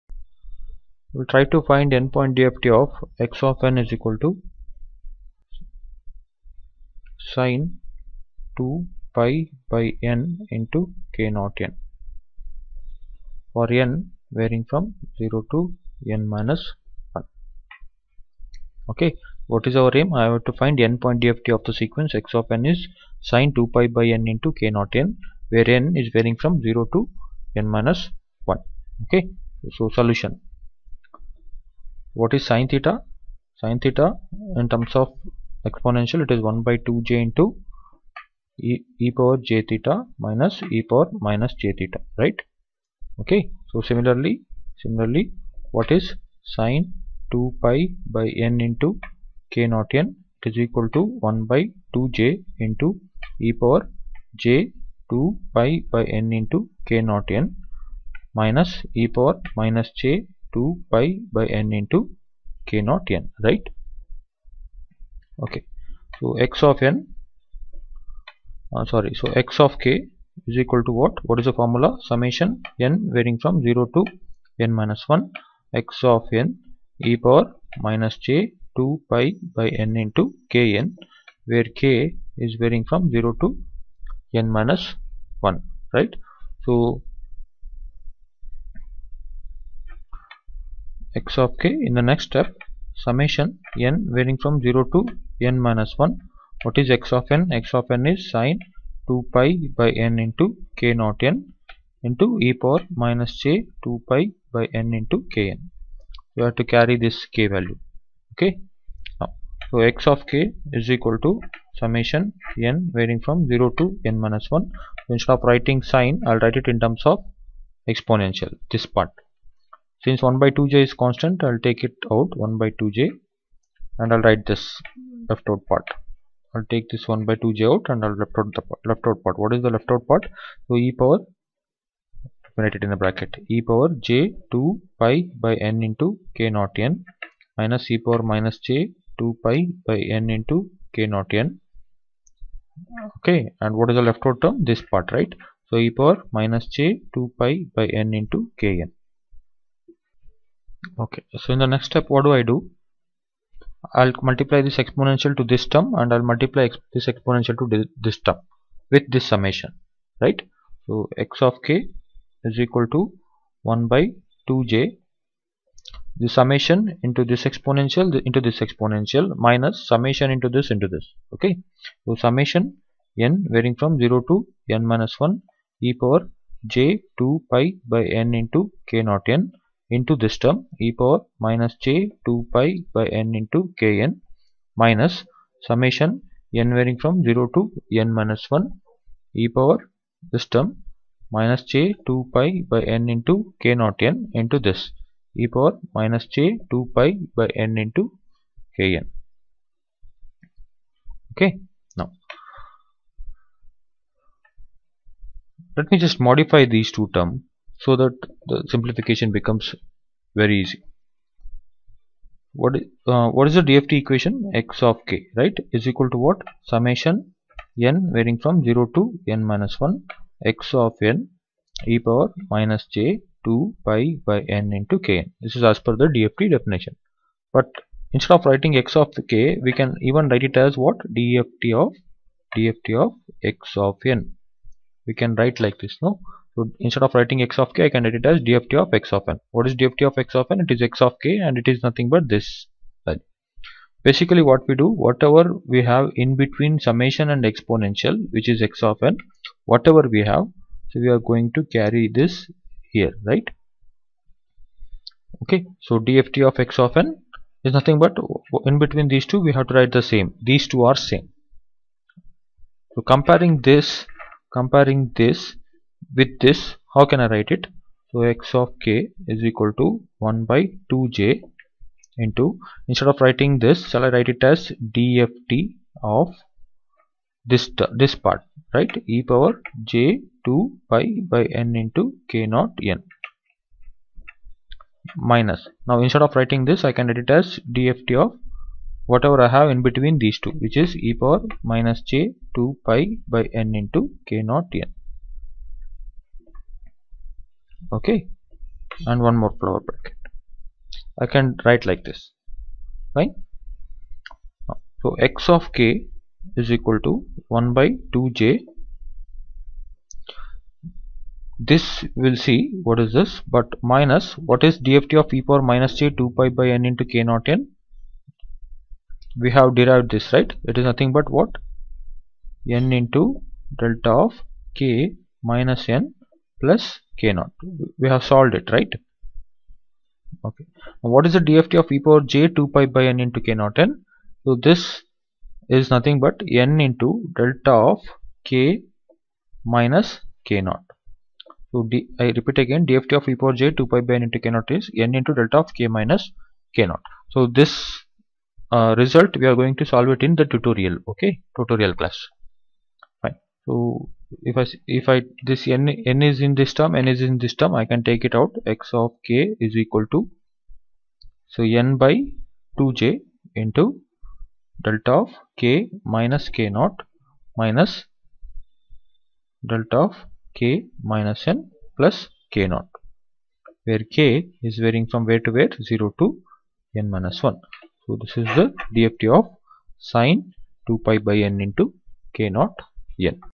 DSP - L6 - Solving N point DFT of a sin function
This problem will give you an idea to solve N point DFT of a sin wave